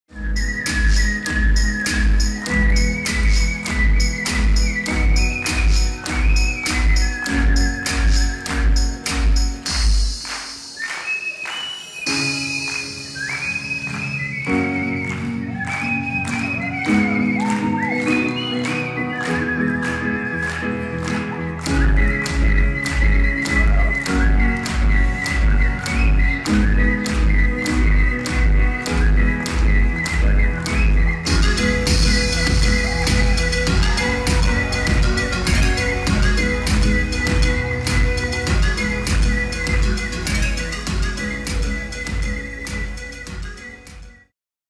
Влада Маринкевич — Художественный свист (День рождения Республики Kids 2019)
3 марта РЕСПУБЛИКА KIDS отметила свой 9 день рождения ярким шоу «Шо-то в этом есть!».